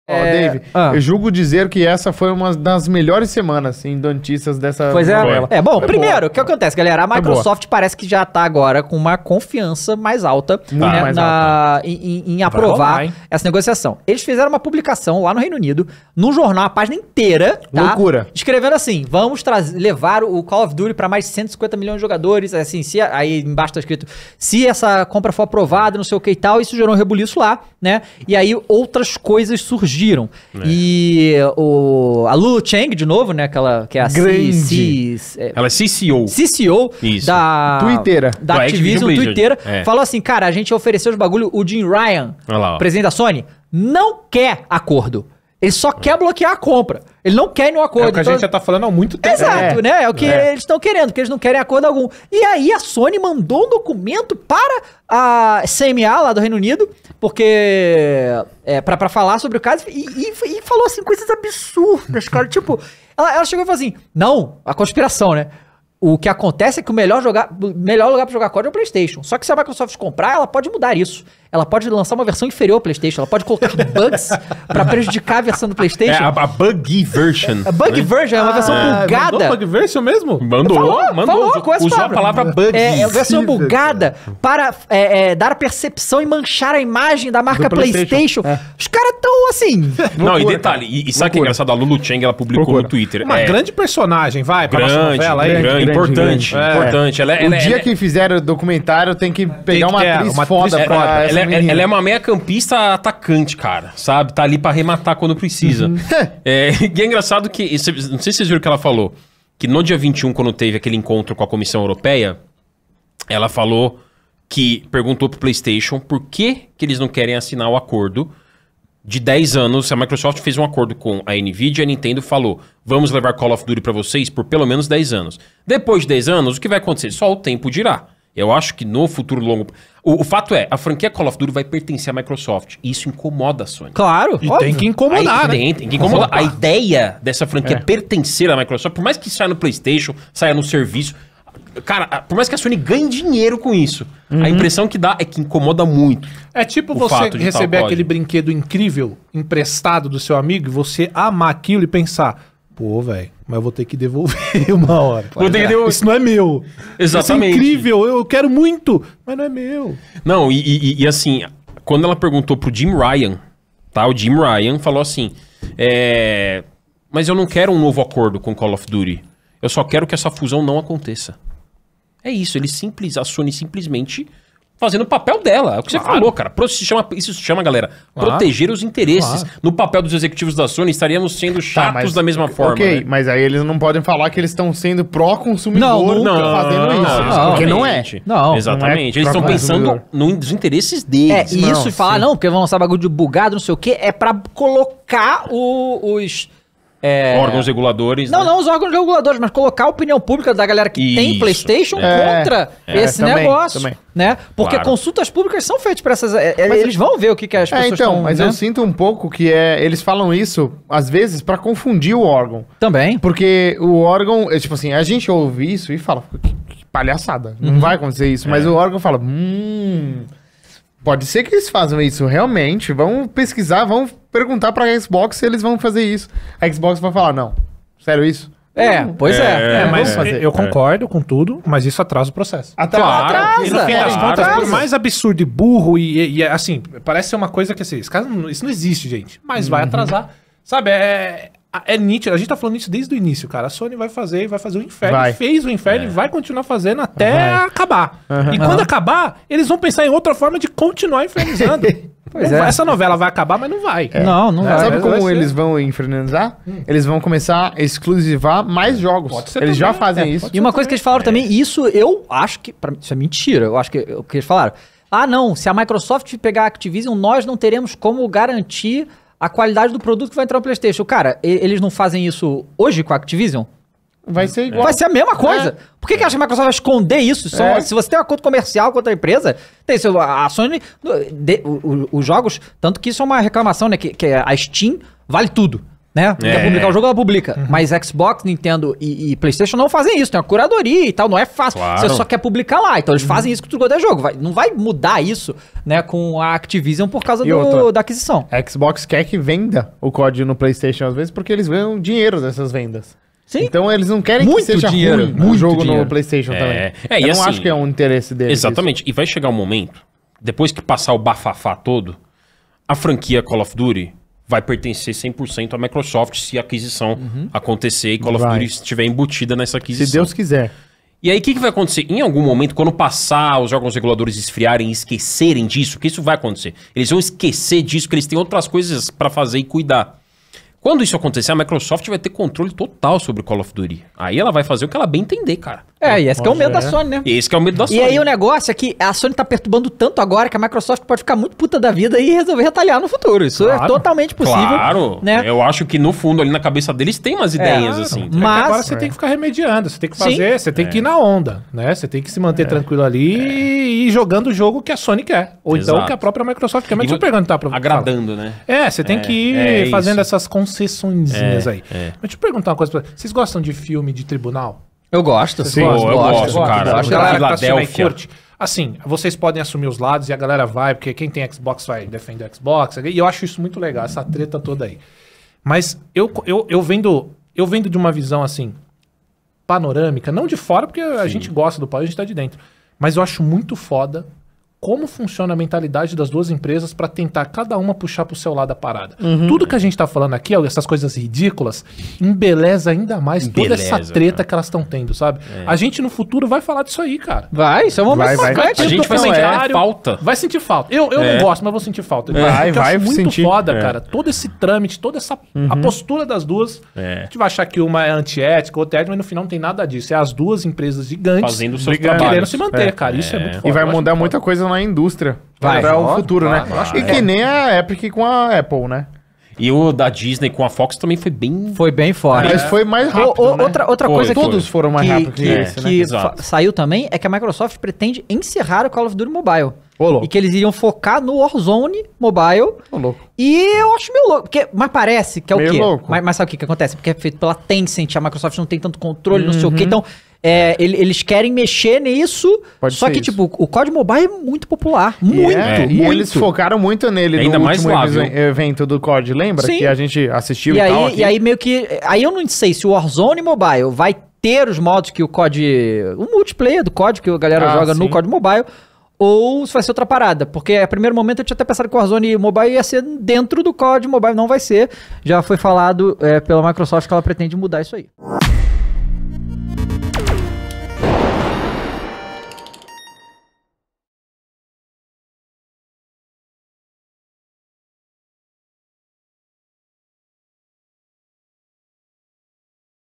Microsoft: CONTROLE TOTAL sobre CALL OF DUTY
Fala galera! No vídeo de hoje teremos um corte comentando sobre como seria se isso acontecesse.